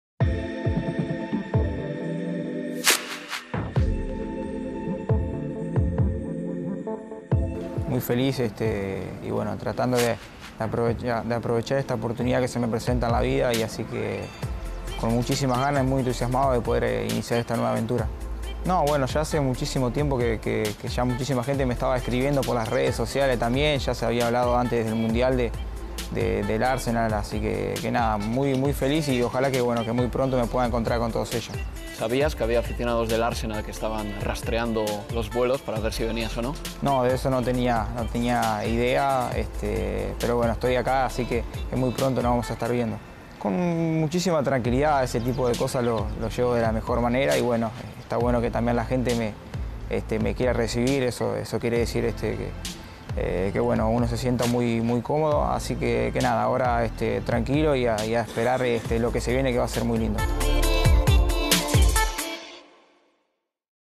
Uruguay's Lucas Torreira 'very happy' at Arsenal move
Arsenal have completed the signing of  Lucas Torreira in a £25million deal. An agreement for the Uruguay international was reached during the World Cup but the deal could not be pushed through until his side had been knocked out of the competition.